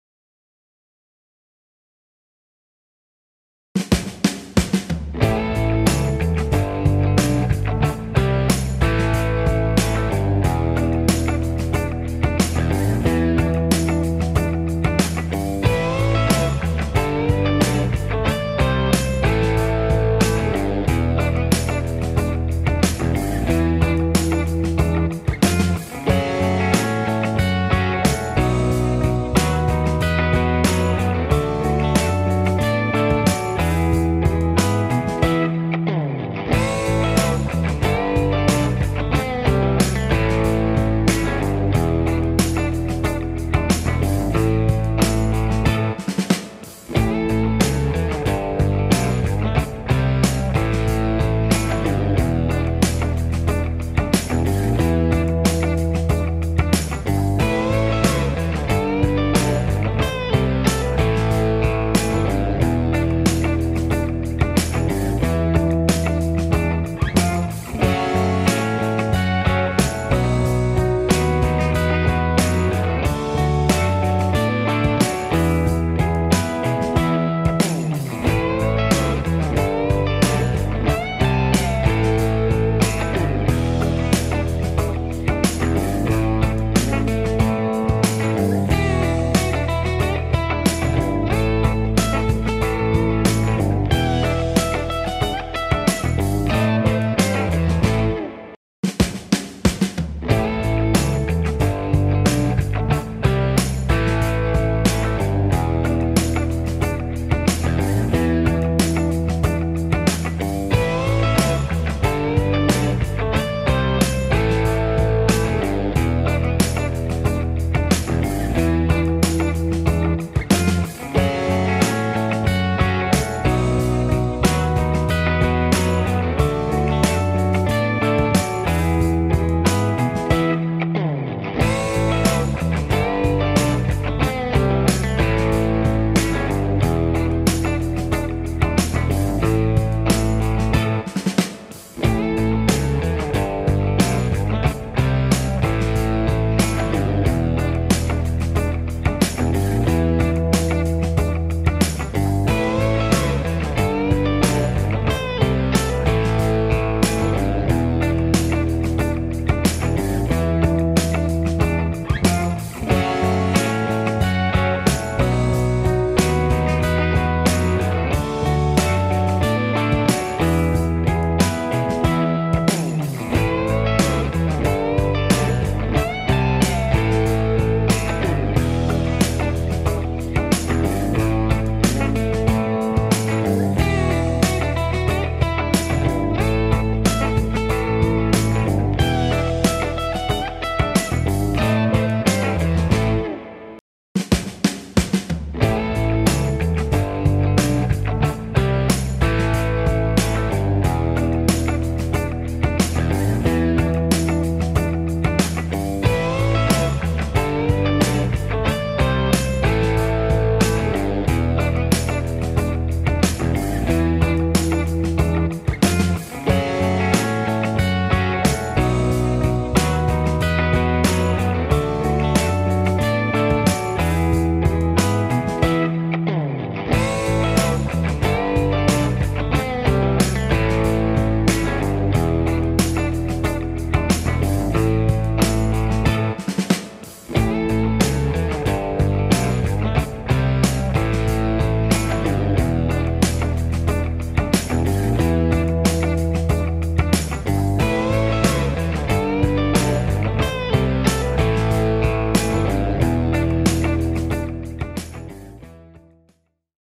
AHREX - Crayfish Zonker - Tied by Håkan Karsnäser
See how Håkan ties this Crayfish Zonker on the NS155 hook from the Nordic Saltwater series of hooks from Ahrex